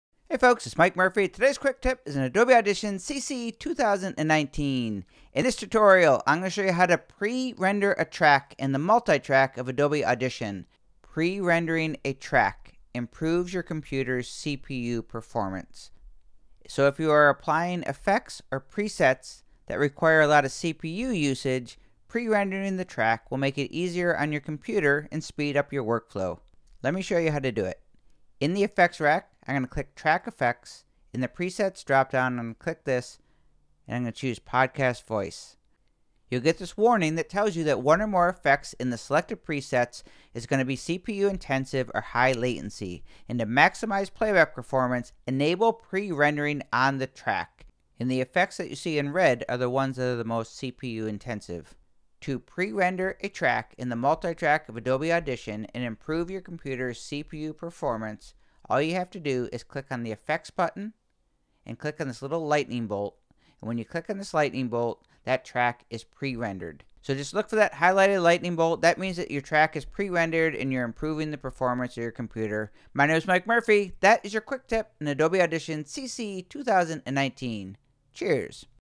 How To Pre-Render a Track in Adobe Audition CC 2019
Pre-Render Tracks in the Multitrack of Adobe Audition CC 2019

This tutorial will show you how to pre-render tracks in Adobe Audition CC 2019

Pre-rendering a track in the Multitrack of Adobe Audition will help the CPU Performance of your computer when using effects that require a lot of computer processing.

1. Add an Effect
2. Click ‘FX’ in the Multitrack
3. Click ‘Pre-Rendering’ button that looks like a lightning bolt
4. The button turns blue when enabled